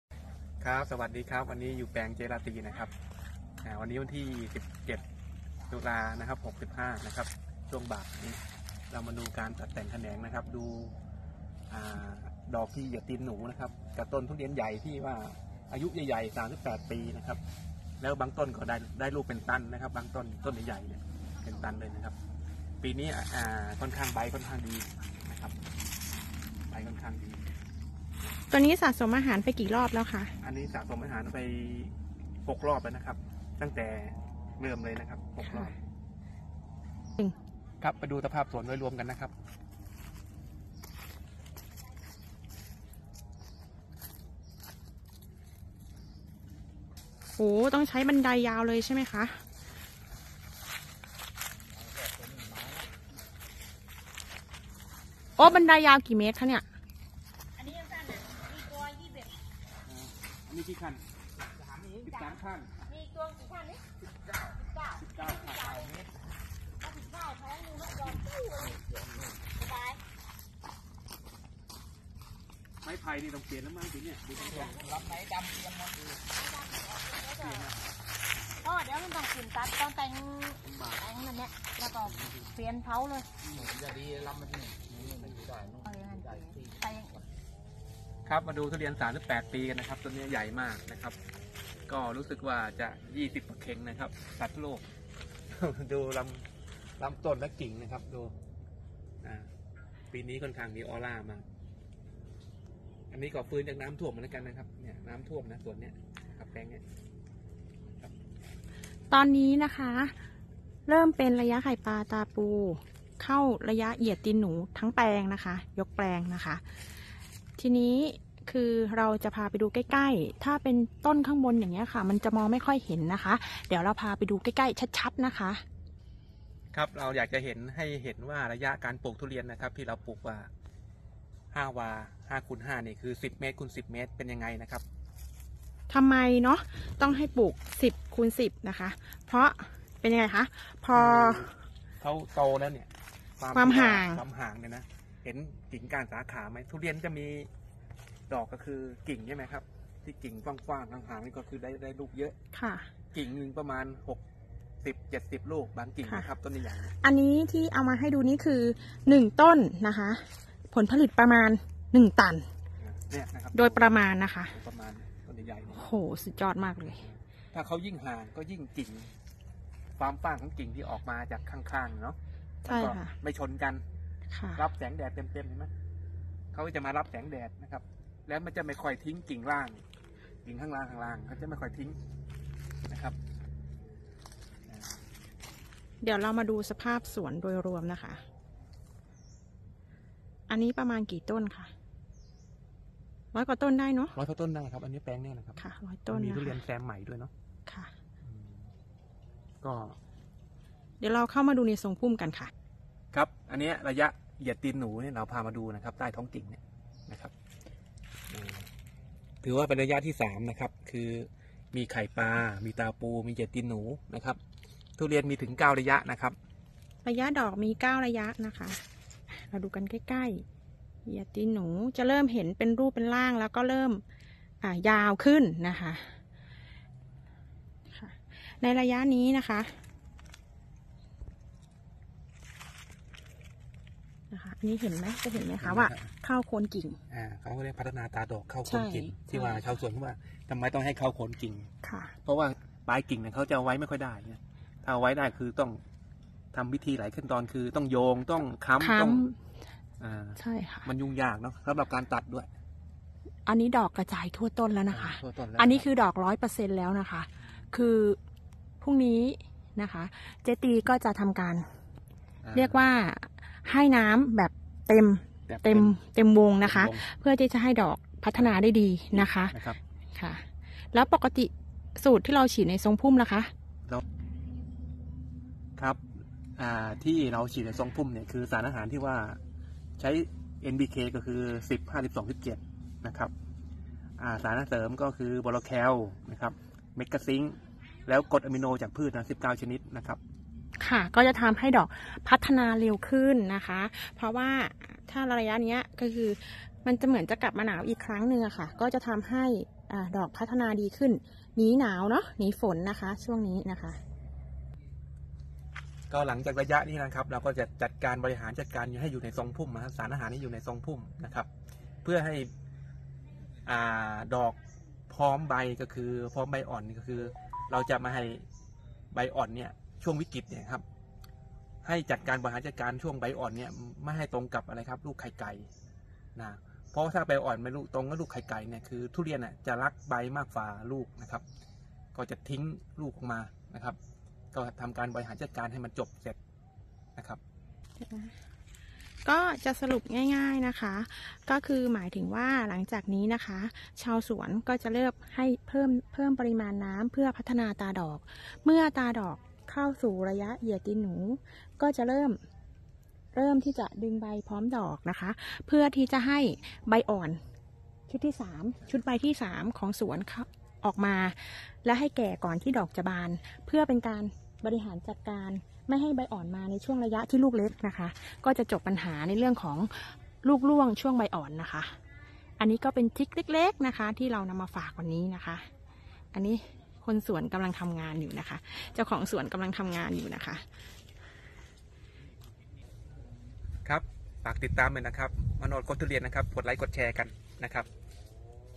เปิดเทคนิคทำทุเรียนอย่างไร ให้ได้ผลผลิต 1000กิโลกรัม /ต้น @CoachManoch | มาโนชโค้ชทุเรียน
ทุเรียน ปี 64 เคยได้ผลผลิตต้นละ 1 ตัน
ปี 65 ดอกพรึ๊บพรั๊บทั้งแปลงแล้วคราฟ📷
ก้าวเข้าสู่
แนวทางของทีม
ลงแปลงตามระยะ ส่งเสริมดอกระยะนี้ให้สมบูรณ์ ยืดขั้วดอกให้ยาวขึ้น ให้ดอกแข็งแรง
ช่งงนี้มีฝนหลงฤดู⛈️
แนะนำ ฉีดกระตุ้นในทรงพุ่มด้วยกรดอะมิโนพืช + แคลเซียมโบรอน + ซิงค์ + NPK ตัวกลางสูง
เฝ้าระวังโรคแอนแทรคโนส(ดอกเน่า)  เชื้อราเข้าทำลายท้องถิ่นทำให้ดอกยุบ ตาดอกฝ่อได้
และเพลี้ยไฟลงดอก ทำให้ดอกเสียหาย
พิกัด บ้านสวนราตรี  อ.แกลง จ.ระยอง
แปลงนี้ ทุเรียนอายุกว่า 38ปี ปลูกระยะ 10×10เมตร
27/10/65
‎@ทุเรียน บ้านเรา
สายด่วน K.มาโนช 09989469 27